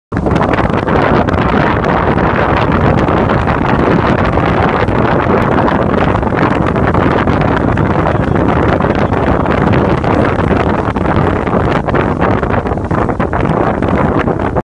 RA1,HI Tech Sustainable Marine in Action(Dubai)
first solar boat in MENA, Dubai FITSOLARTECH.COM
11knots cursing speed,The Future Now